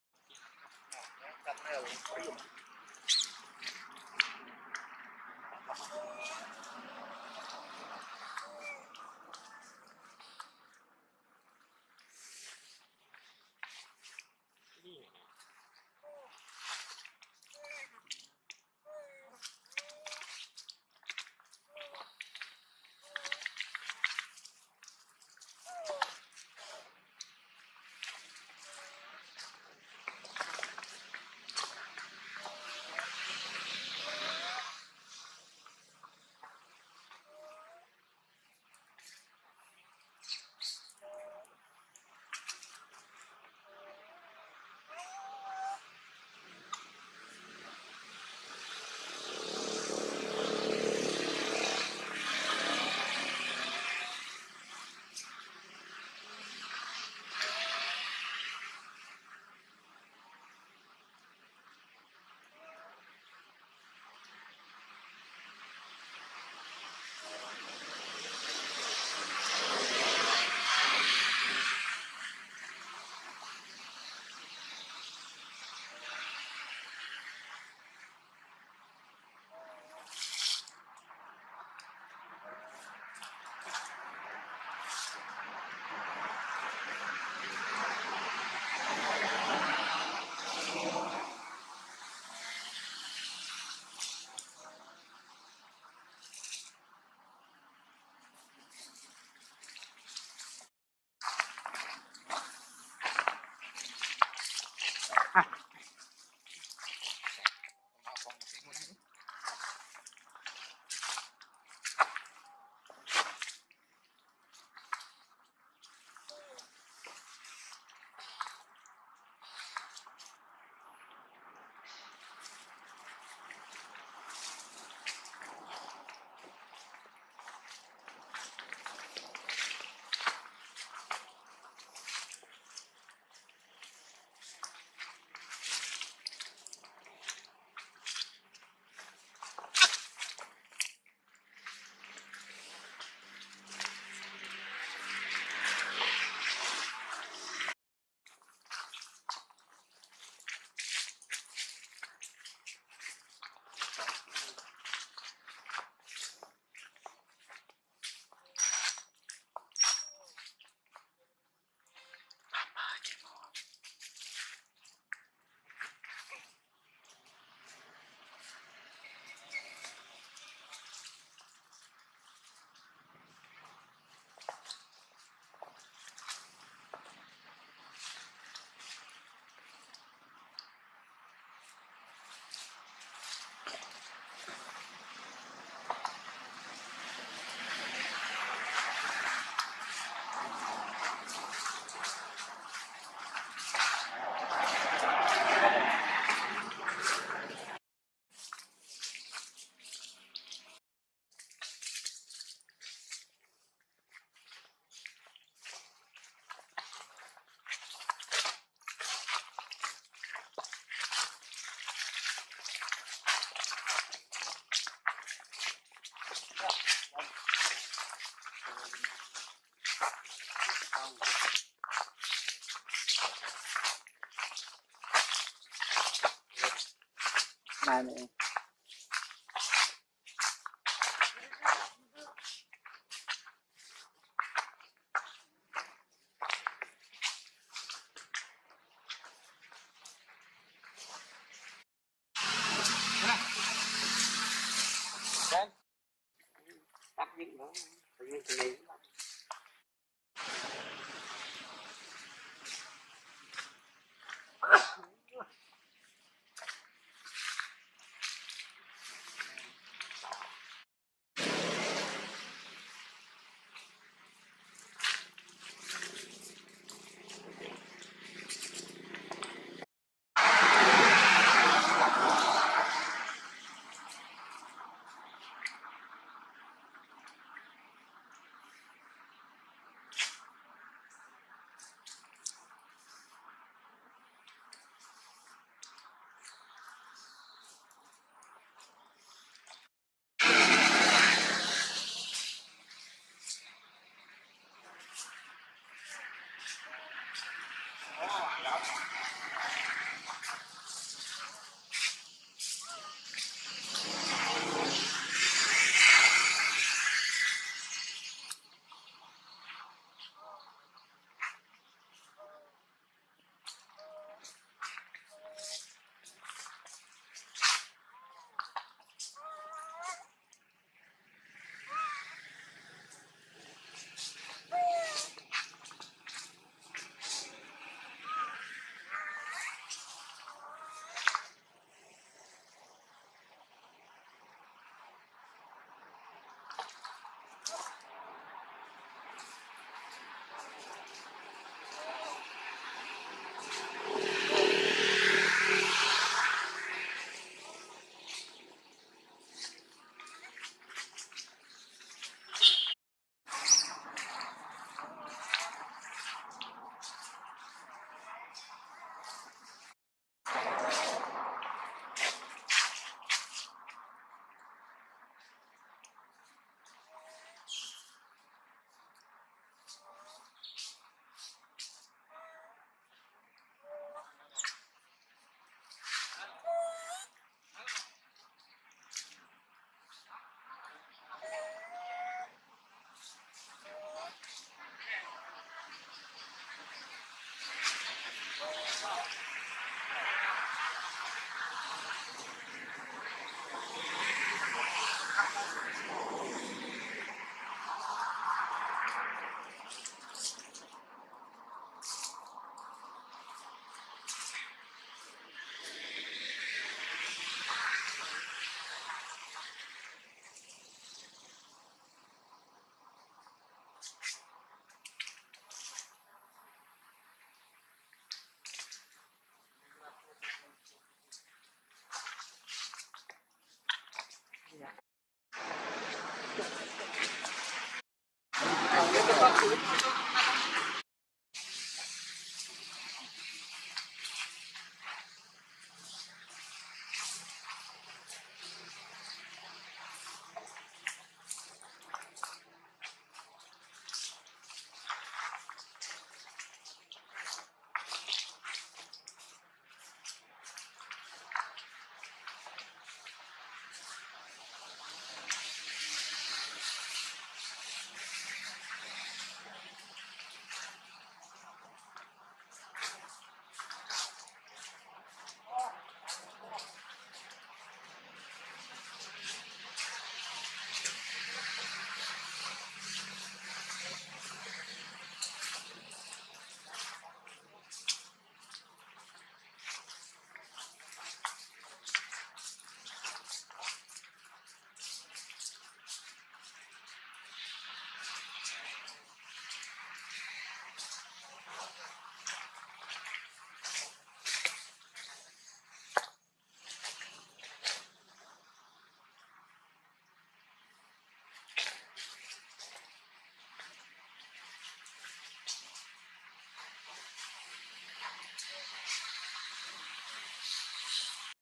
BRAT JANNA MOANS TO ASK FOOD FROM TOURIST, SHE MOANS MANY TIME BUT NOTHING FOR HER
Sincerely yours,
BB Monkey Comedy
Category
Pets & Animals